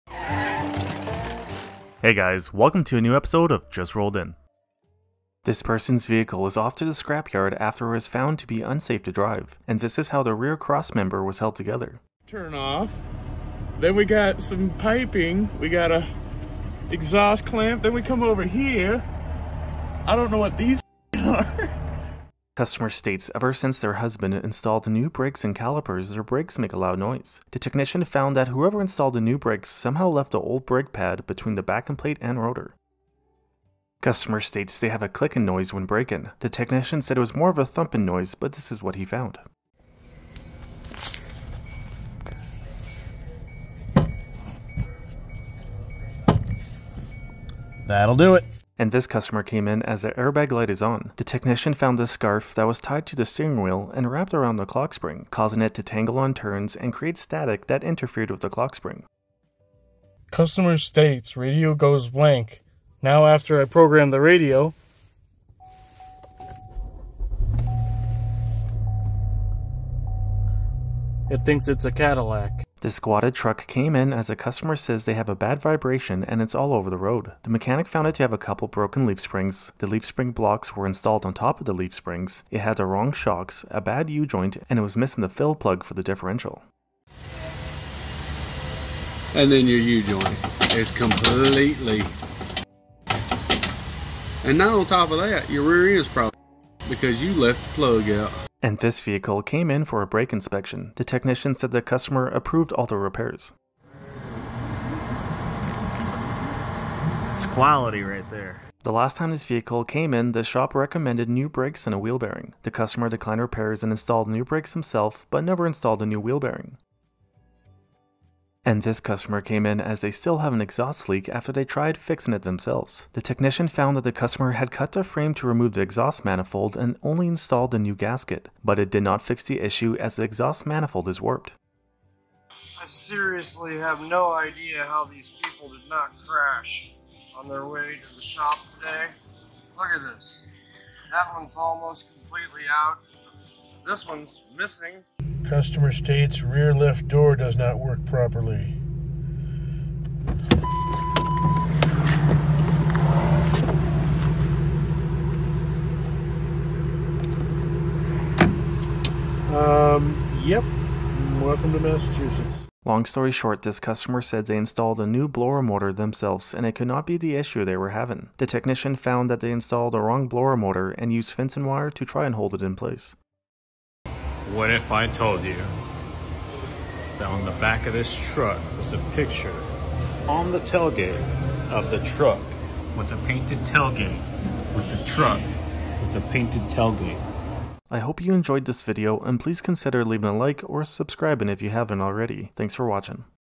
Customer States Another Mechanic Said Their Car Is Junk | Just Rolled In
0:00 Intro
0:05 They mentioned it was an early 2000s Chrysler (did not remember what model). But luckily it's no longer on the road!
0:22 Somehow her husband got an old brake pad stuck behind the new rotor and backing plate. Not sure how he did it, but that was the cause of the noise.
0:34 "2016 Kia Soul. Video pretty much speaks for itself. Tho it was more of a "thumping" noise than a "clicking" noise but that's what the customer said."
0:48 "I believe that is a religious (Hindu) scarf tied to the steering wheel and the customer decided to wrap it around the clockspring to secure it, causing it to tangle on turns and create static. I actually untied it at first and did the diag to confirm the issue...then I re-tied it for the video lol. The customer just purchased it 300 km / 186 miles (3 weeks) ago from our used car dept and has decided to go ahead and replace the clockspring for tomorrow."
1:00 "2020 Buick Enclave fixed after TAC technical assistance center sent special program code after waiting for 3 weeks for their engineering team to come up with a solution."
1:12 The customer declined repairs to get any of those issues fixed. The rear shocks were also the stocks that came with the lift kit (as the front is only lifted), so they were bottomed out. The rear differential was low on gear oil as well.
1:38 Front brake rotors were worn down to the cooling vanes, worn out lower ball-joint and brake hoses pinched off. The technician did not mention the whole list of repairs that it needed but they got all of them done.
1:48 Not sure why the customer did not install a new wheel bearing when they installed new brakes. However, the worn-out wheel bearing on the passenger side destroyed the new brake rotors, caliper bracket, and brake caliper. They will need a new cv-axle and spindle now as well.
1:58 Shop refused to work on the truck as the customer had cut the frame on it. The technician said it needed a new exhaust manifold, gasket, and manifold studs.
2:12 One bolt for the upper control arm is missing and the other one is loose and almost all the way backed out. Seems to be a common issue on these Chevrolet Trailblazers from what the technician said.
2:25 Around 4 gallons/ 15.1 litres of coolant was added to the engine.
2:38 No extra info.
2:55 2008 GMC Sierra 2500HD 6.6L 230k km.
"The customer came in for no defrost, we found faulty mode and temp actuator. Then noticed a lack of airflow. The customer said they replaced the blower motor themself and it couldn't be the issue. They ground off the housing to install the wrong fan used fencing wire to fix it and then taped off around it. Installing the proper fan and fixing damages to the HVAC case solved the issue."
3:06 Tailgateception.
3:20 Outro. Thanks for watching!